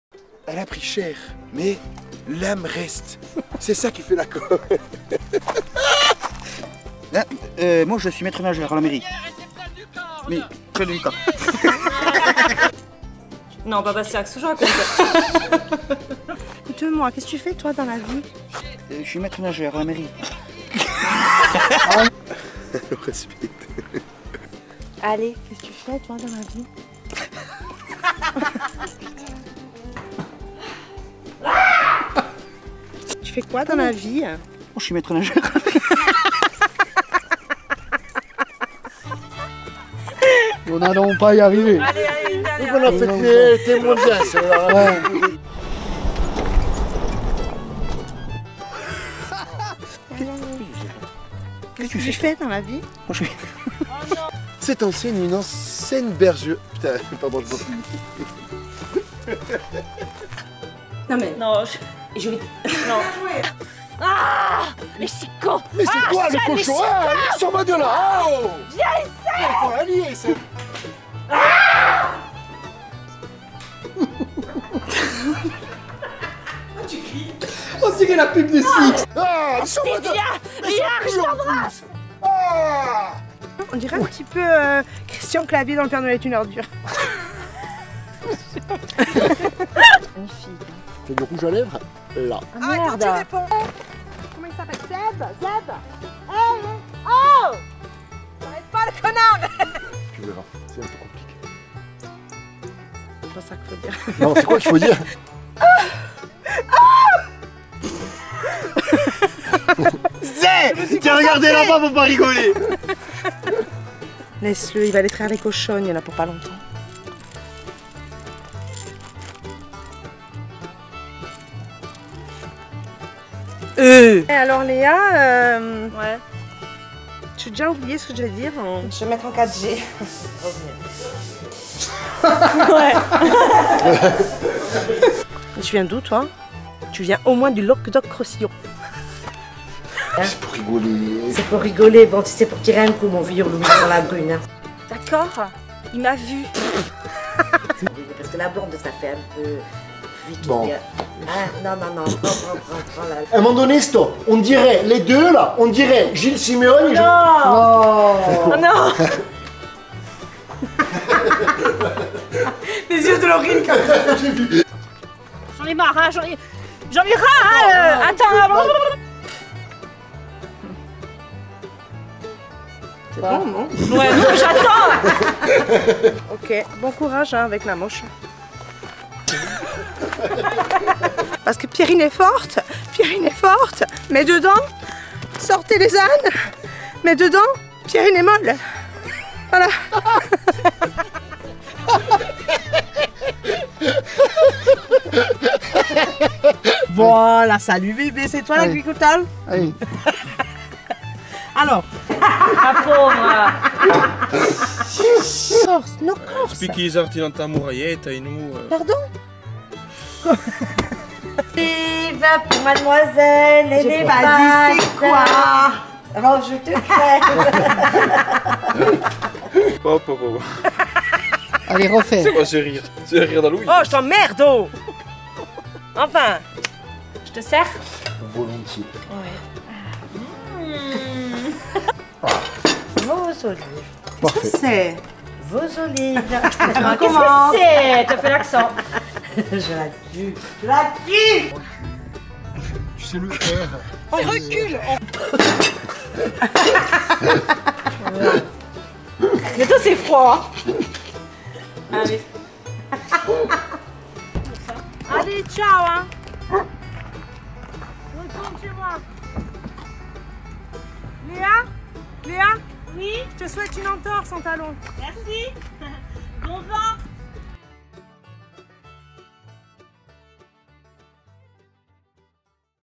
I Kongoni - L'Amour est dans la machja - Le bêtisier
Il fallait bien finir sur une nouvelle note de rires. Voici le bêtisier de cette aventure !